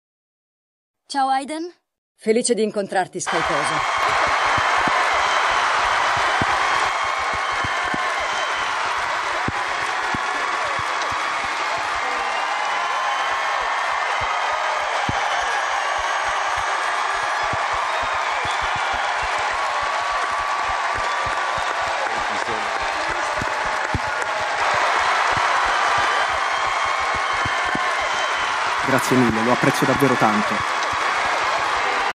Hayden Christensen incontra Ashley Eckstein per la prima volta in italiano
Breve clip tratta dalla Star Wars Celebration del 2022 in cui la doppiatrice di Ahsoka Tano Ashley Eckstein incontra l'attore di Anakin Skywalker Hayden Christensen per la prima volta.

Per doppiare il video in italiano ho utilizzato le voci generate tramite intelligenza di Erica Necci e Francesco Pezzulli, rispettivamente i doppiatori dei due personaggi interpretati da Ashley e Hayden.